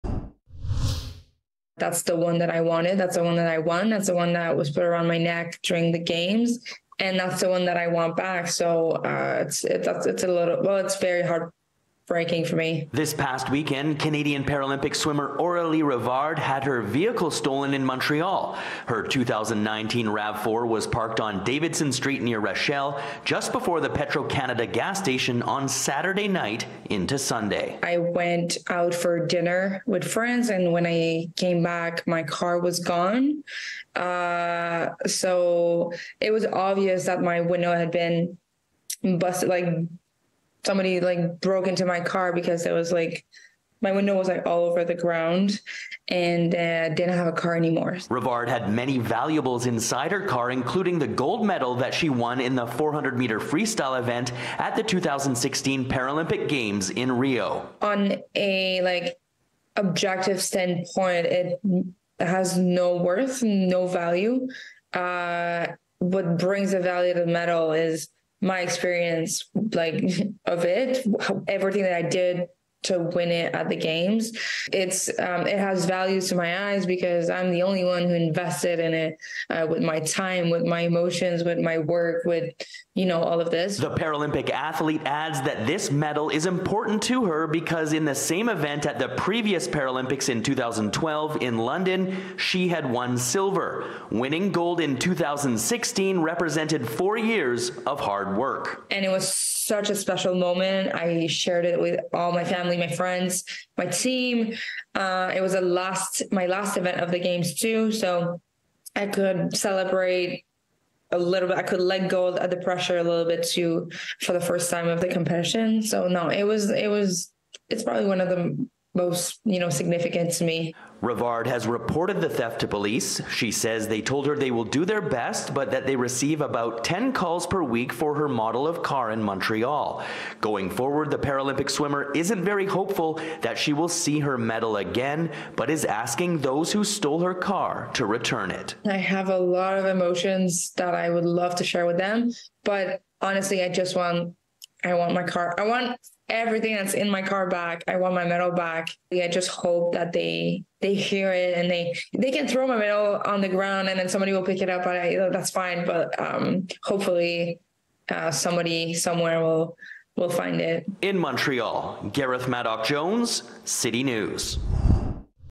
Paralympic gold medal stolen in Montreal car theft
“It's very heartbreaking for me,” said Canadian Paralympic swimmer Aurélie Rivard, about her 2016 Paralympic gold medal that was stolen in a Montreal car theft. Gareth Madoc-Jones reports.